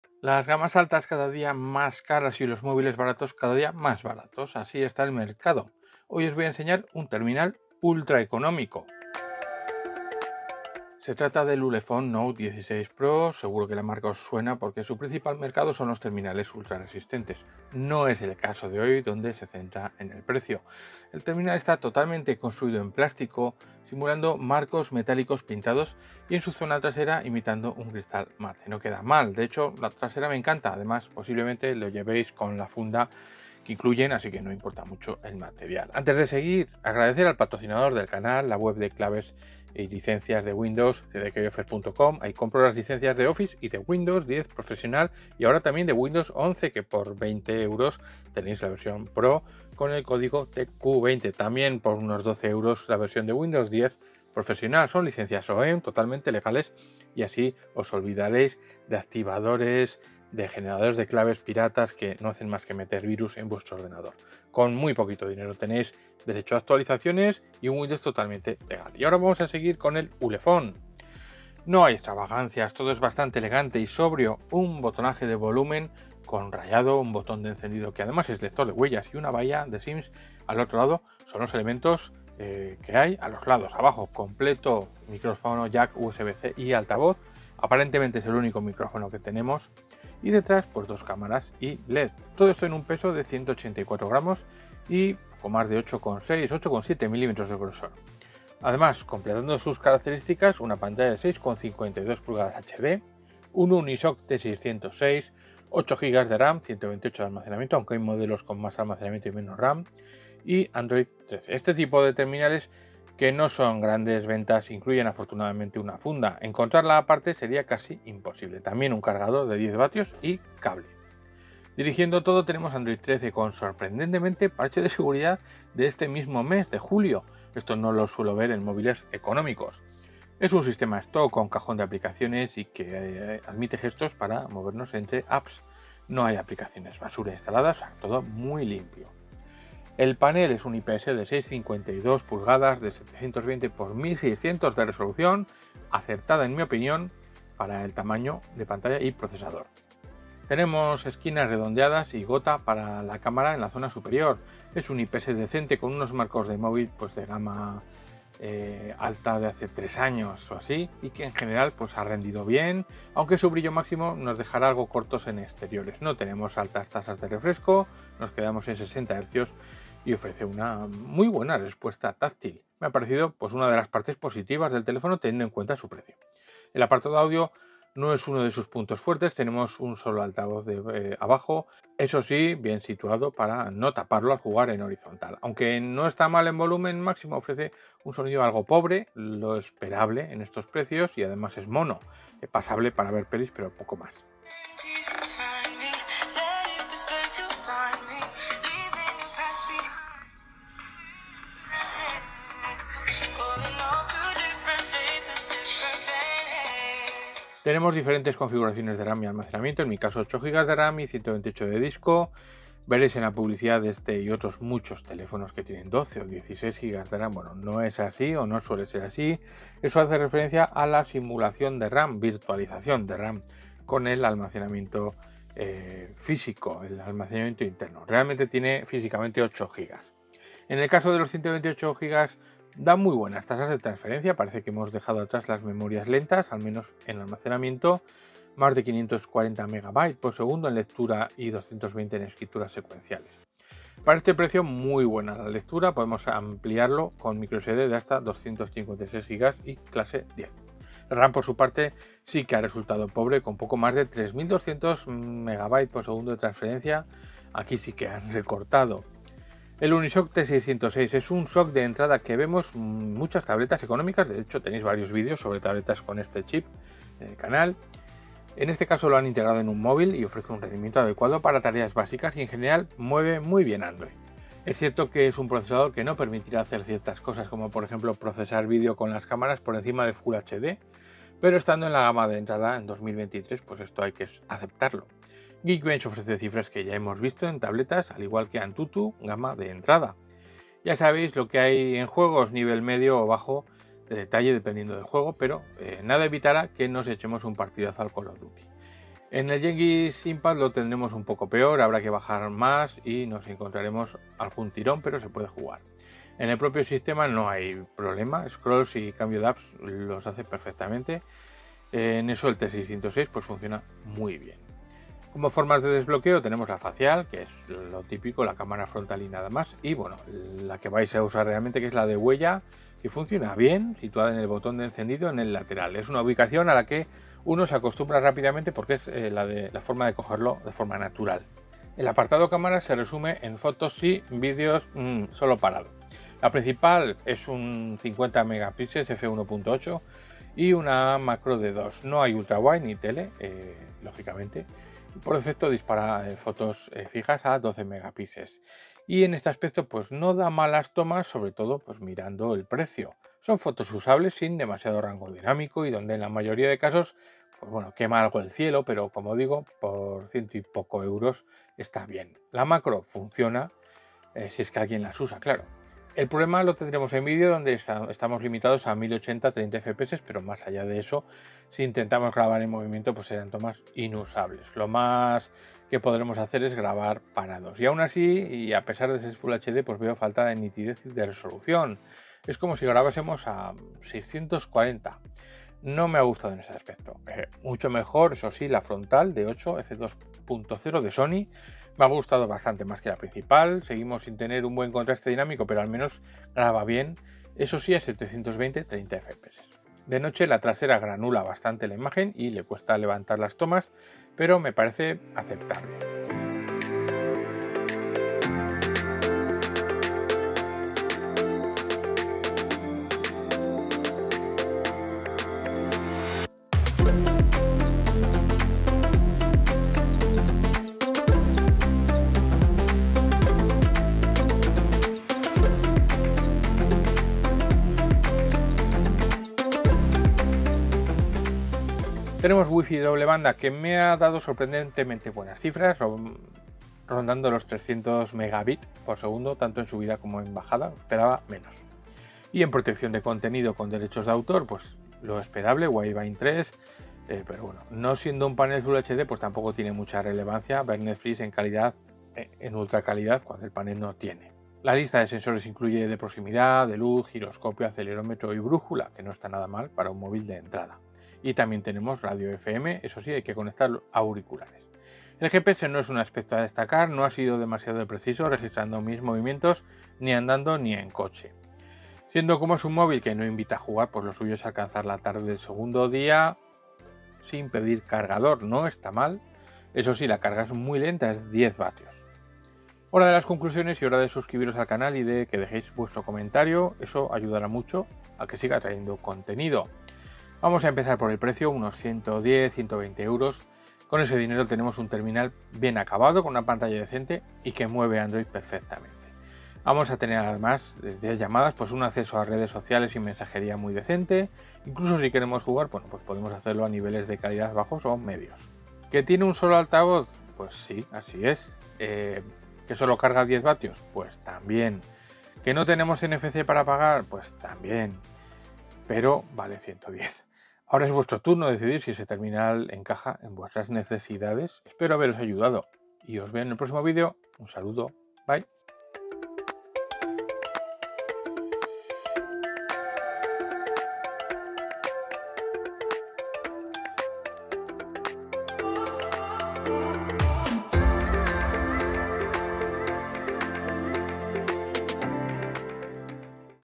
Qué puede hacer un teléfono de SÓLO 123€ ? ULEFONE NOTE 16 PRO
Cada vez más baratos, así son los terminales de entrada asiáticos que llegan.

👉Licencias software:
 Código 25%: TQ20